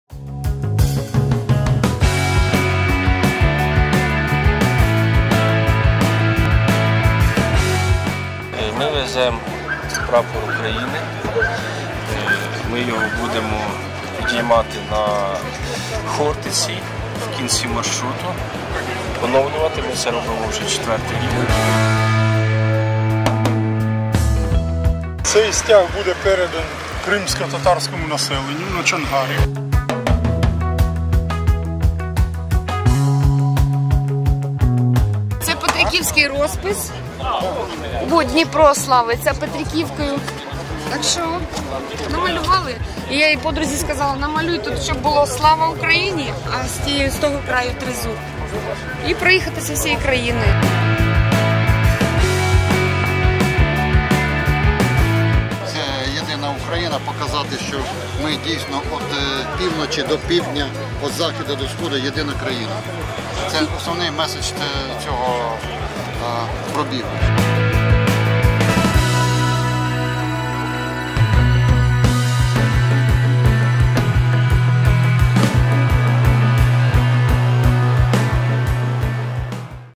У Тернополі побували байкери "Мотопробігу Єдності 2020"
Проїхати 54 міста з українською символікою - така мета "Мотопробігу Єдності 2020", яка  об'єднала байкерів з усієї України. Тернопіль - одне з них. Як це було і заради чого відбувається - дивіться у нашому відео.